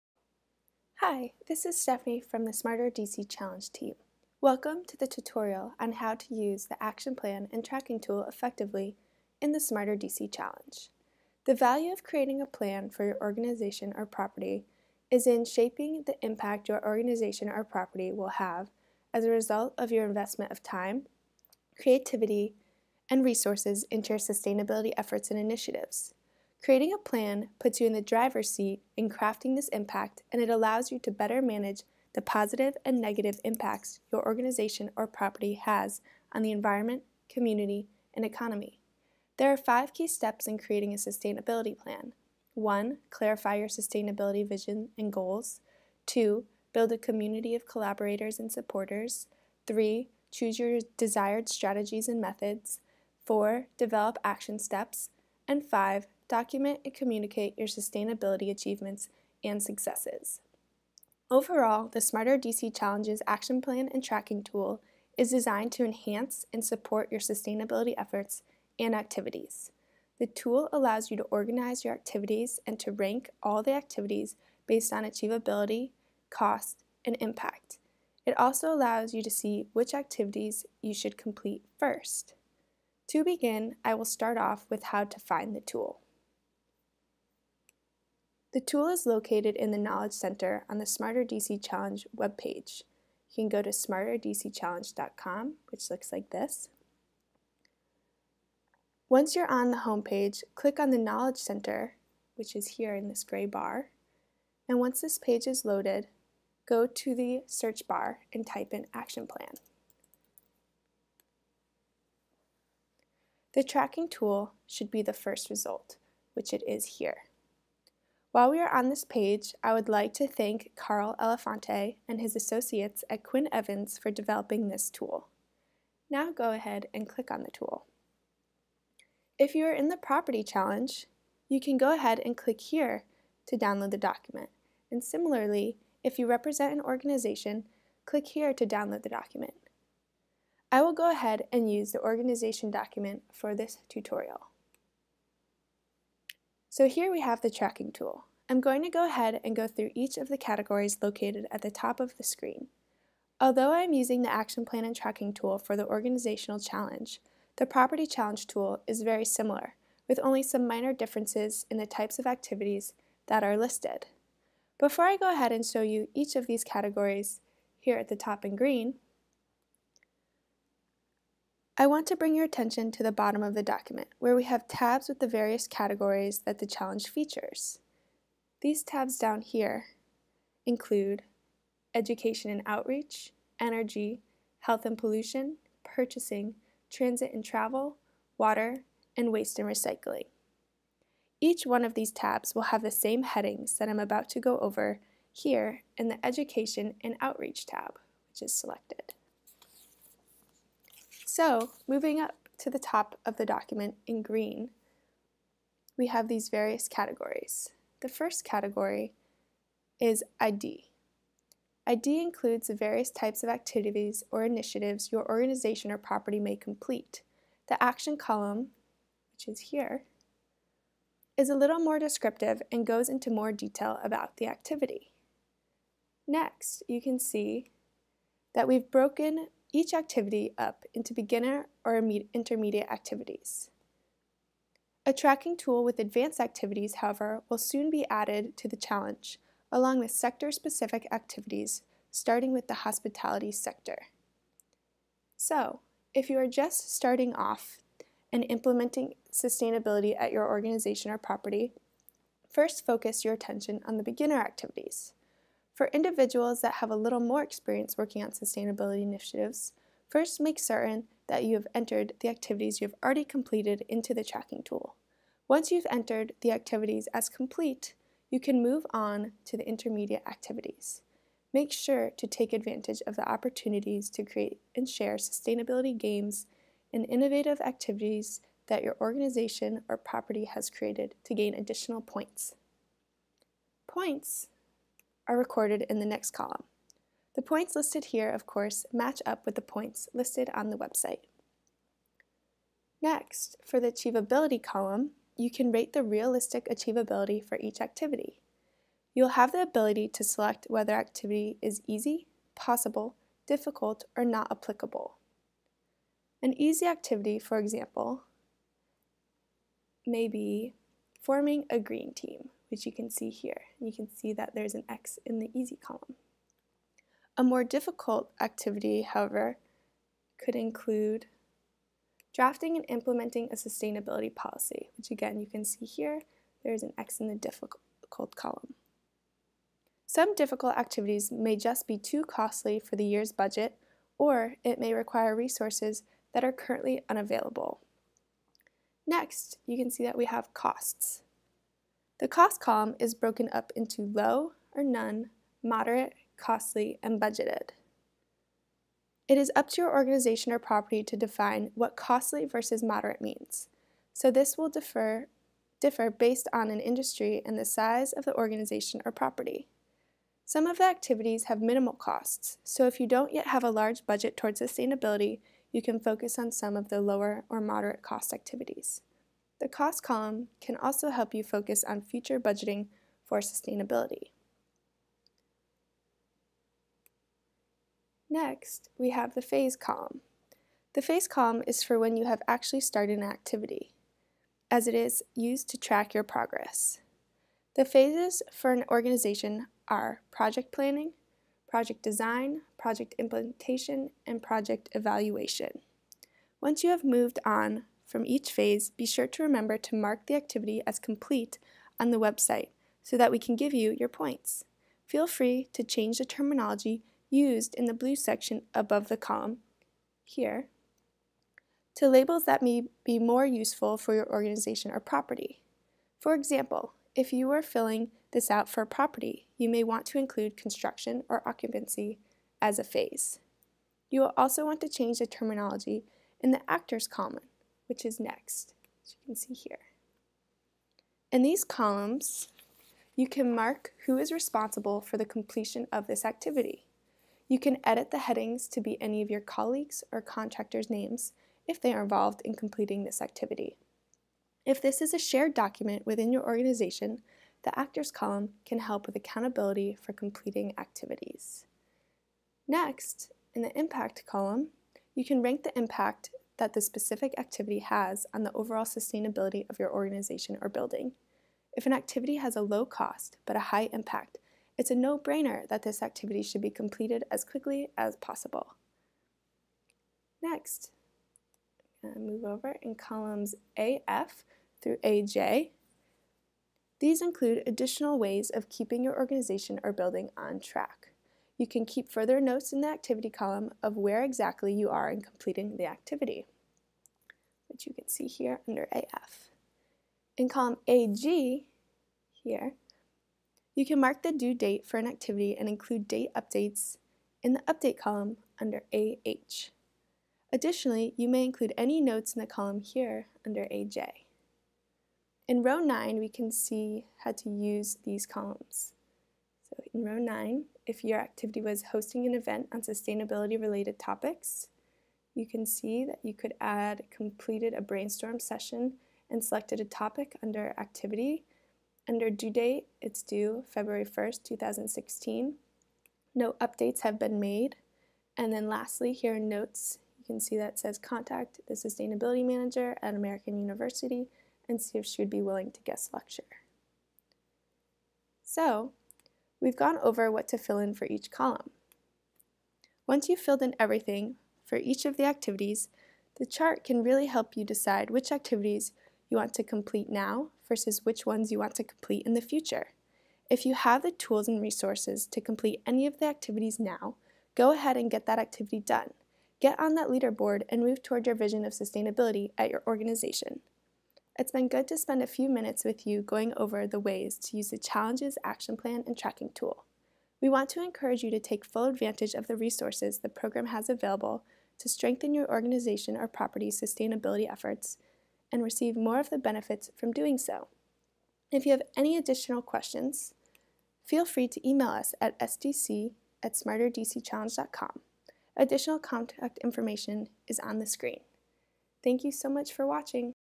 How to Use the Action Plan & Tracking Tool
Have you signed up for the Smarter DC Challenge, and would like to track activities outside of the online tool before entering completed activities into the tool? The Action Plan & Tracking Tool was designed to help you organize your activities and to rank all the activities based on achievability, costs and impact. It also allows you to see which activities you should complete first. Watch this short informative tutorial on how to use the Action Plan and Tracking Tool.